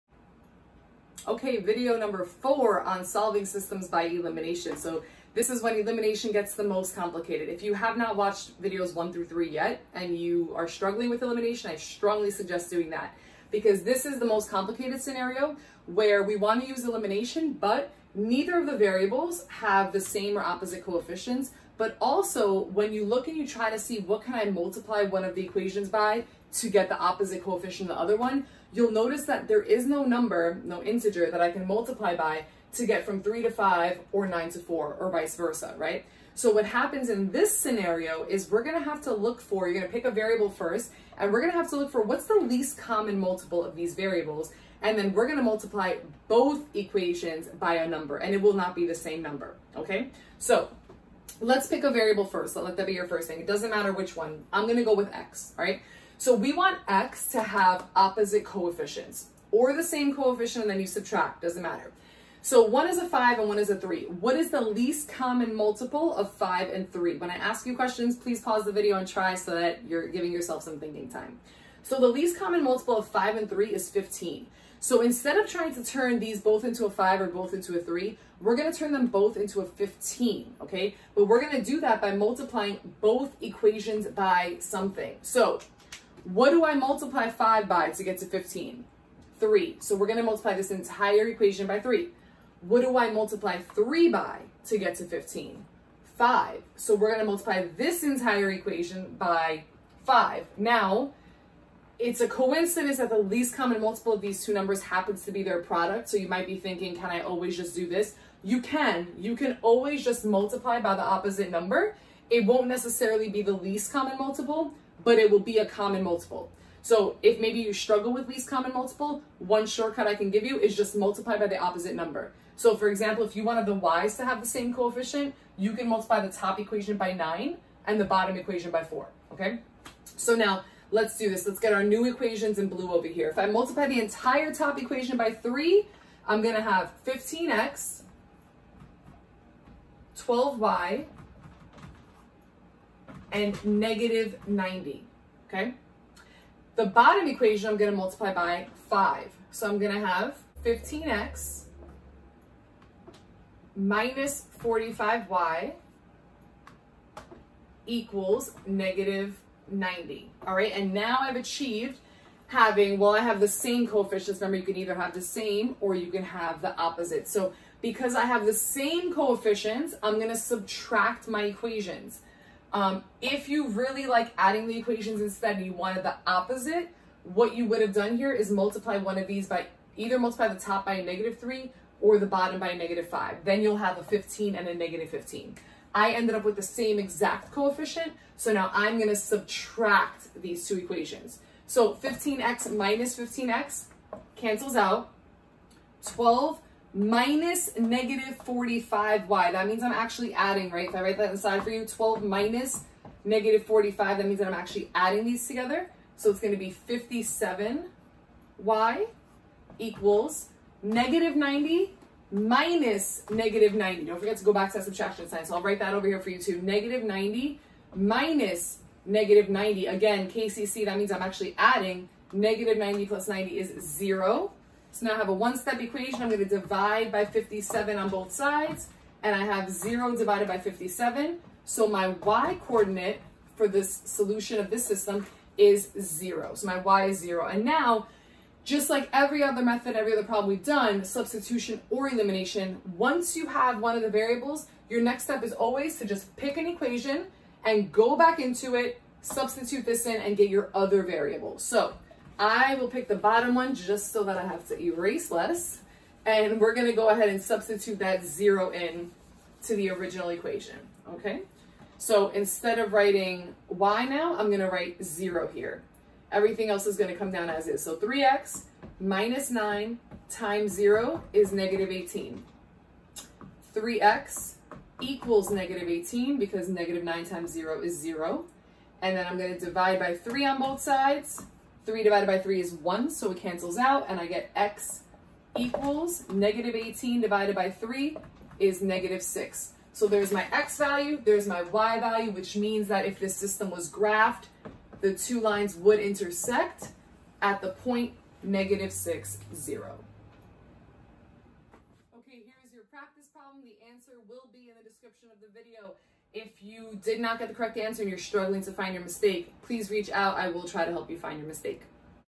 Elimination (Part IV: Multiplying both equations)
This video explains how to solve a system by elimination when neither coefficient of either variable is a multiple of the other. In this case, we will multiply BOTH equations to get a common factor as the coefficient.

The solution for the practice problem is (-2,-4)

HIT THE THUMBS UP BUTTON IF YOU GOT THE PRACTICE PROBLEM CORRECT!